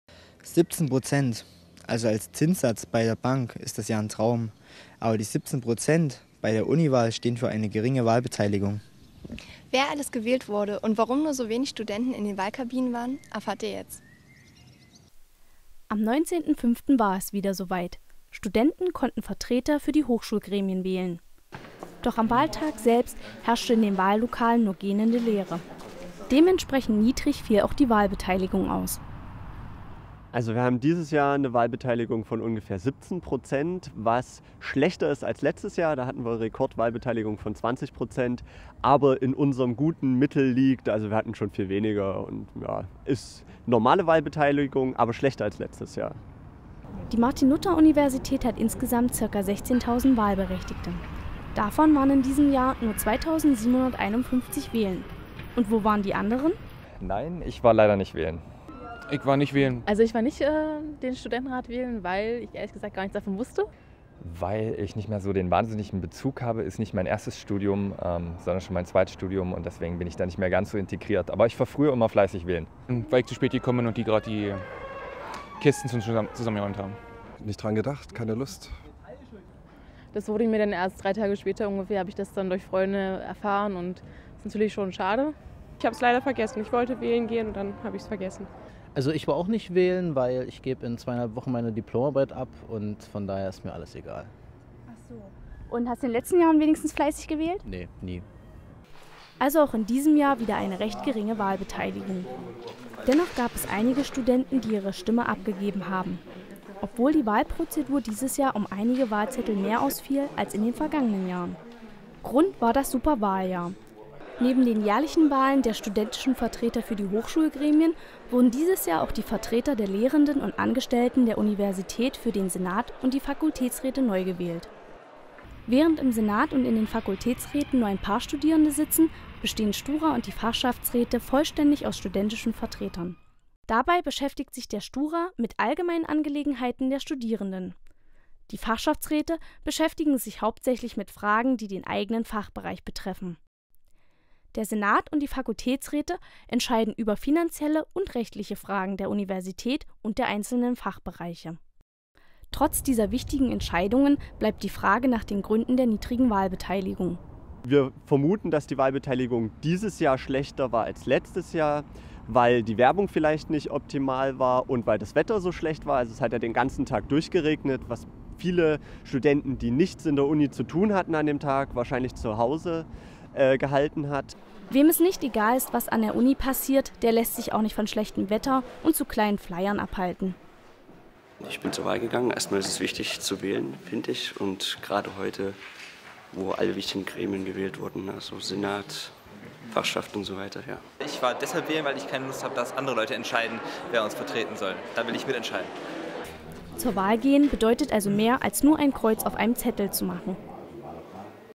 24.Sendung - Uniwahl
Großes Wahljahr an der Martin-Luther-Universität - kleine Wahlbeteiligung. Warum eigentlich?